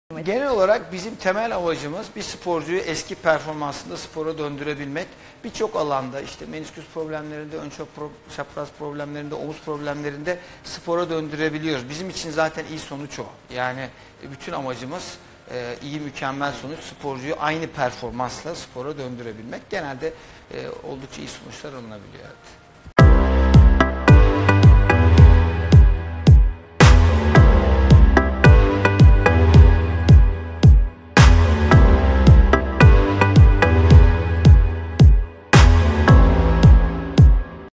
Sakatlıktan Sonra Spora Devam Edilebiliyor Mu? - DOÇ. DR. Ertuğrul AKŞAHİN
Hafta içi her sabah saat 7.30'da, Hilal Ergenekon ve Dr.Onur Kulaksızoğlu'nun sunumlarıyla, sağlığa dair her konu TV8 ekranlarında!